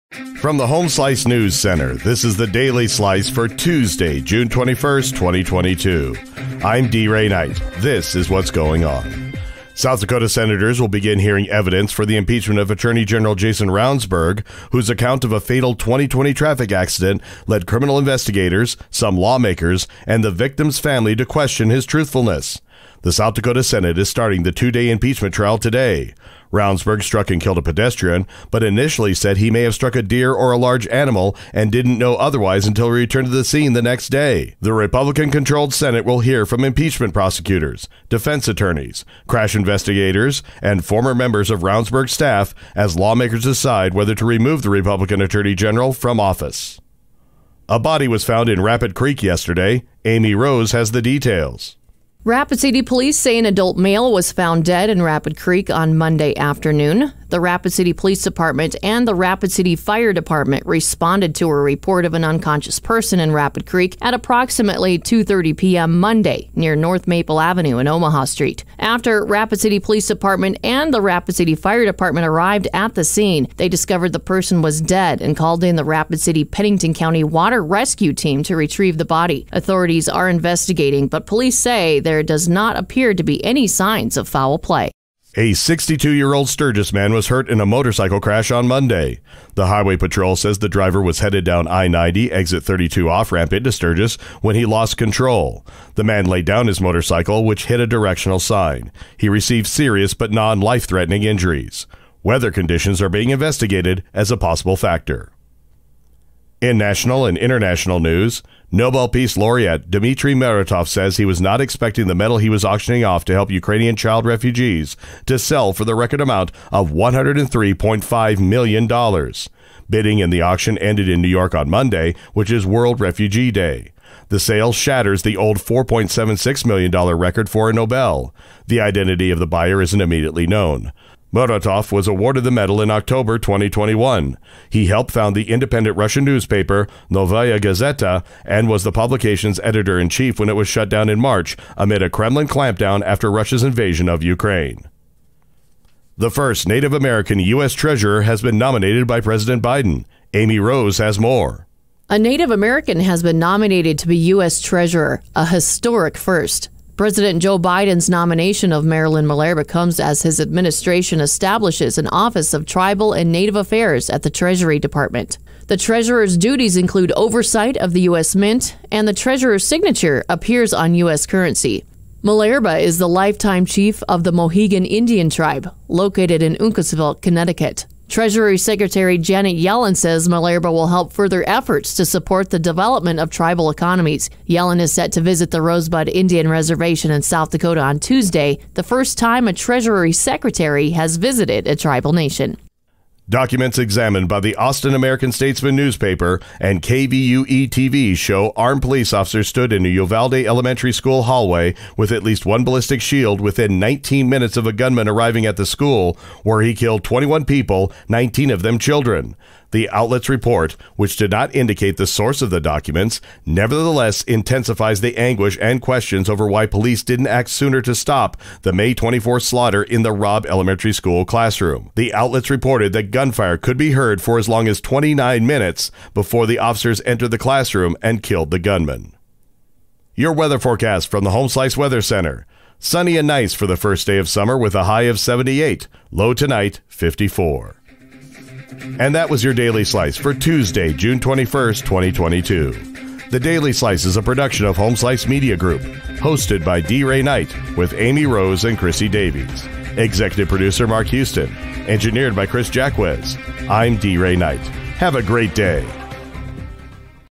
Daily Slice for Tuesday, June 21, 2022
Ravnsbourg impeachment begins, Body found i Rapid Creek, Sturgis motorcycle crash, Record for Nobel auction, First native american treasurer, New info on Uvalde police response. weather